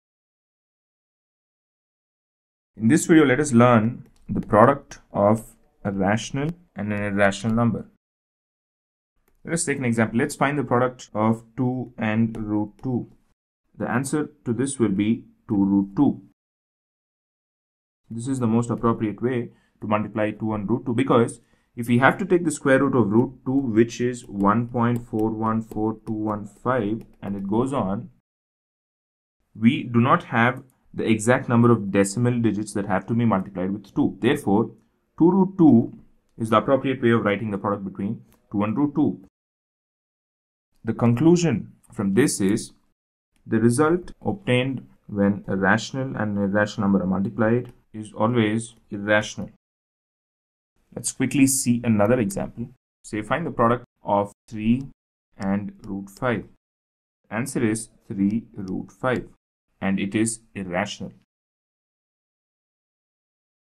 How to multiply a rational number and an irrational number?
We sure know multiplication but what about multiplication of a rational number and an irrational number? We'll find out how in this video!

This video offers a simple explanation on Product of Rational Number and an Irrational number and what form the answer is in.